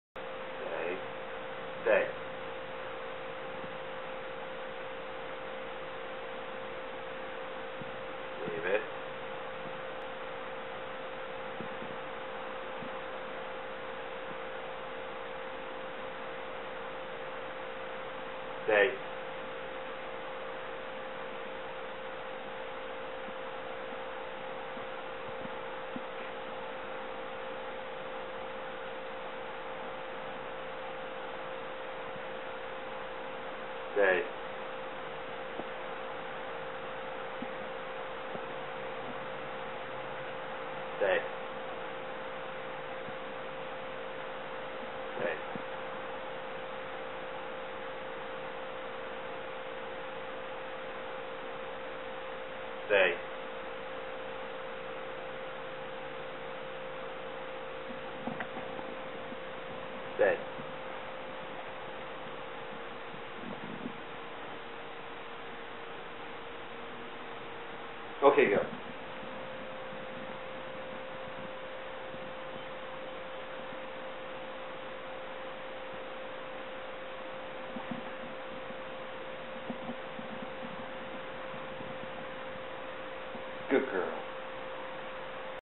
PetStew.com: Dog Stays While Owner Covers Her in Ham
Owner teaches Chesapeake Bay Retriever dog to hold still, covers her in ham to tempt it, then releases the dog to eat the ham.  Funny!!!  See more cool dog tricks and learn how to teach them to your dog